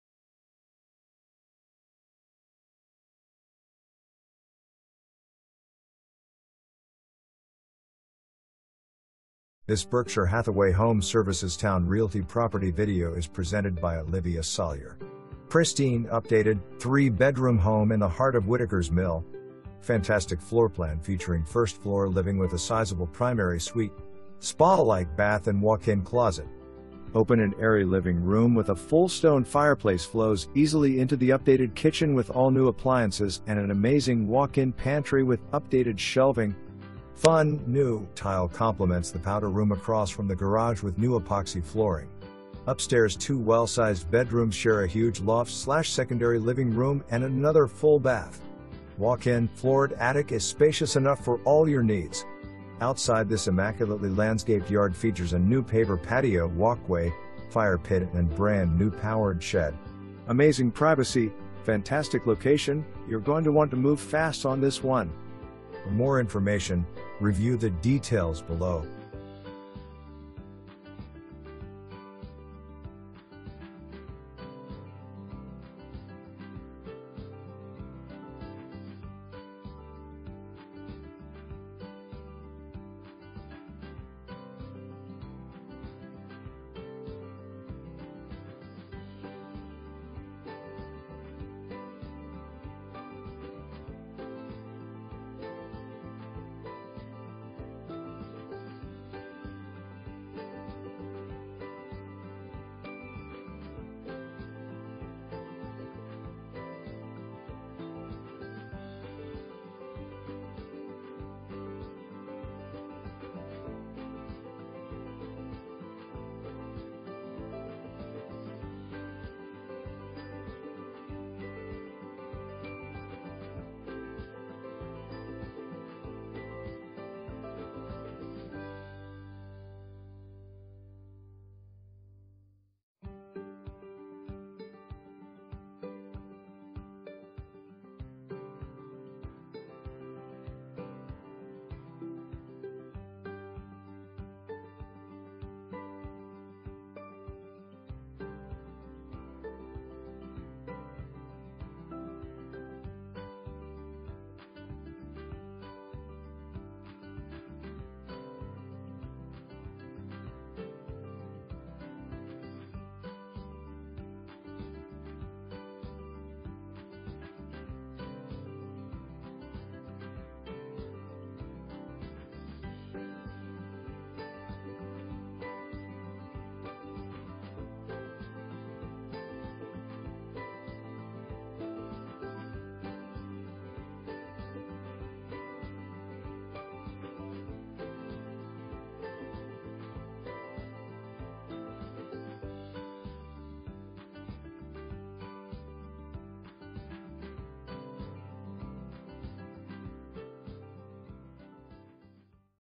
Homes for Sale - 514 Clements Mill TRCE, York County, VA
514 Clements Mill TRCE
York County, VA 23185

Pristine, updated, three bedroom home in the heart of Whittaker's Mill! Fantastic floorplan featuring first-floor living with a sizable primary suite, spa-like bath and walk-in closet. Open and airy living room with a full stone fireplace flows easily into the updated Kitchen with all new appliances and an amazing walk-in pantry with updated shelving! Fun, new, tile compliments the powder room across from the garage with new epoxy flooring. Upstairs two well sized bedrooms share a huge loft/secondary living room and another full bath. Walk-in, floored attic is spacious enough for all your needs! Outside this immaculately landscaped yard features a new paver patio, walkway, fire pit and brand new powered shed! Amazing privacy, fantastic location, you're going to want to move fast on this one!

2 full bath, 1 half bath

Olyvia Salyer
Berkshire Hathaway HomeServices Towne Realty